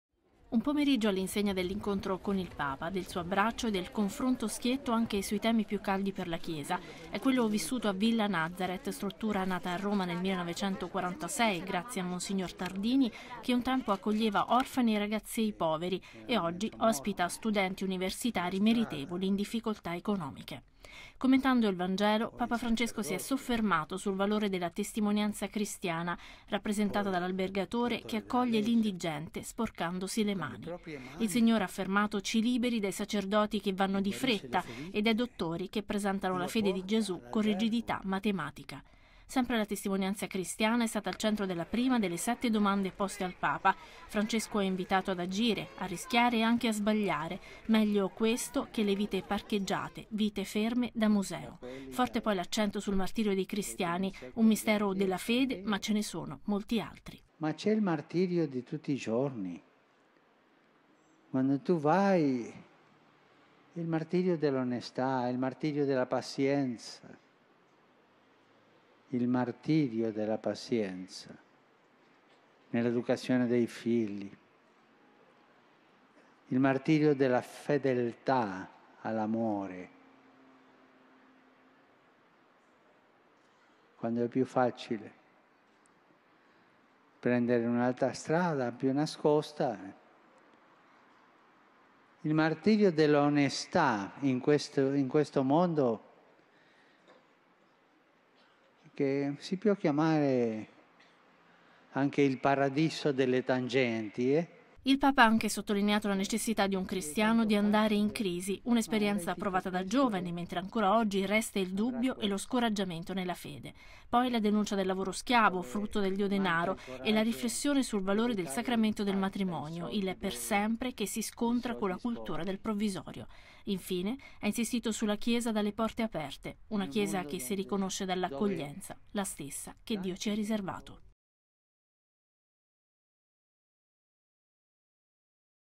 Papa visita Villa Nazareth 18-06-2016
Il Papa a Villa Nazareth: il martirio dell’onestà nel paradiso delle tangenti

Papa Francesco ha visitato Villa Nazareth, struttura romana che ospita studenti in difficoltà economica. A loro ha ricordato che la testimonianza cristiana è uno sporcarsi le mani.